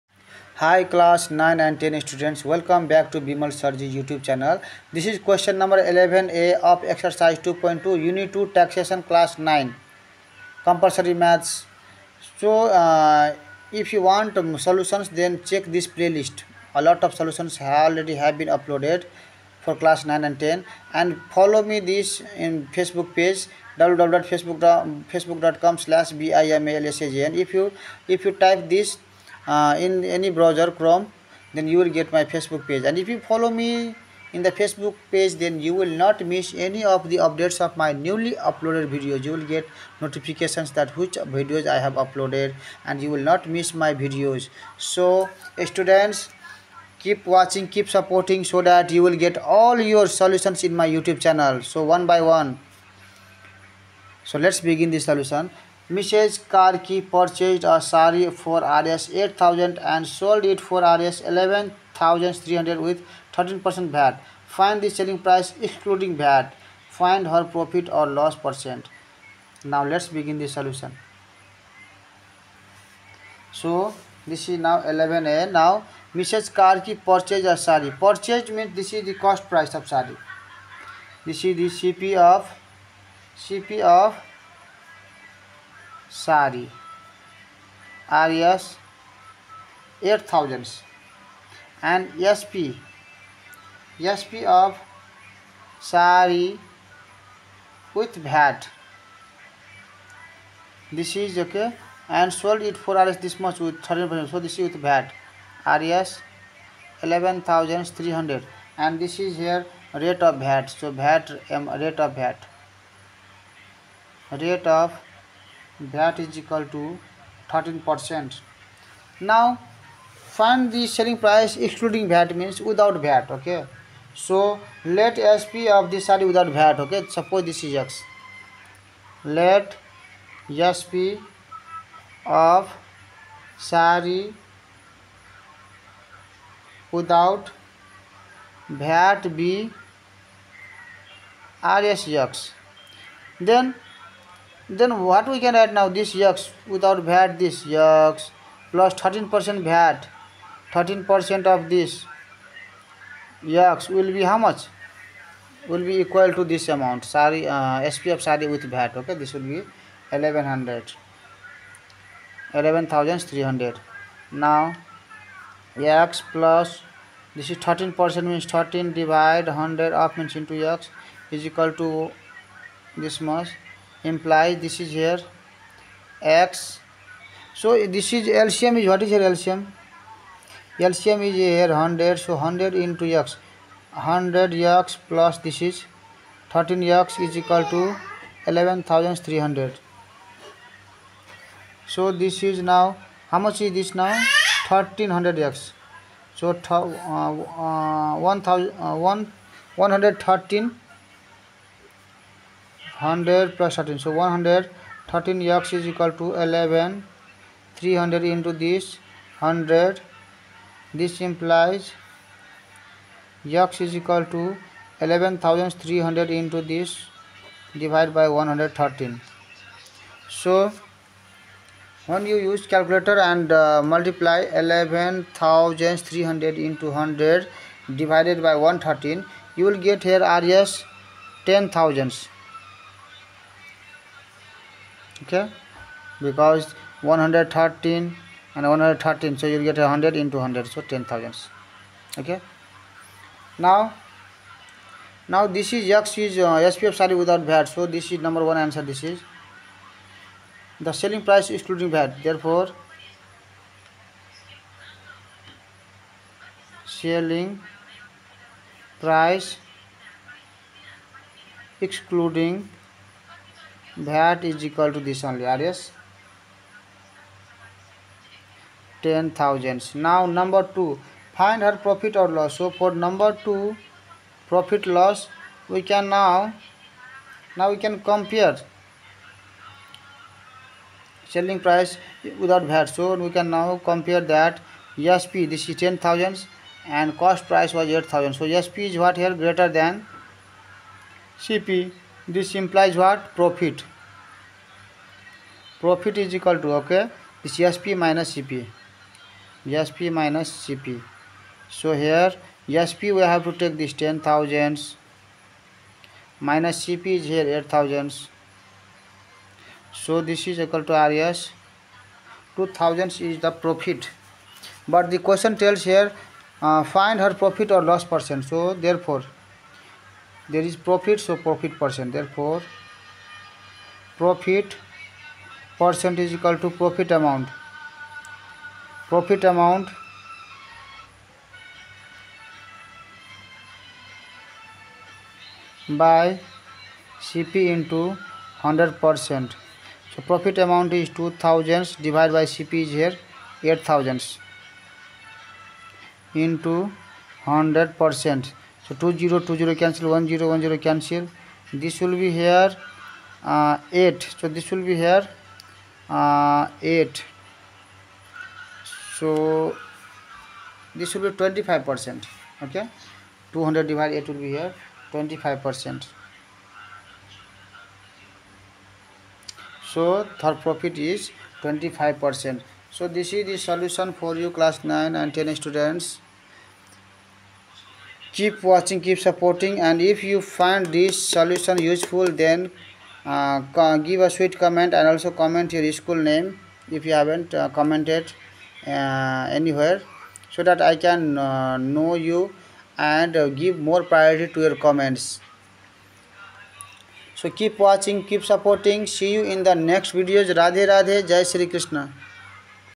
11a) Mrs. Karki purchased a sari for Rs 8,000 and sold it for Rs 11,300 with 13% VAT.(i) Find the se
11a) Mrs. Karki purchased a sari for Rs 8,000 and sold it for Rs 11,300 with 13% VAT.
(i) Find the selling price excluding VAT.
(ii) Find her profit or loss percent.

Class 9 Taxation  chapter exercise 2.2 Creative Section Vedanta publication solutions maths science tricks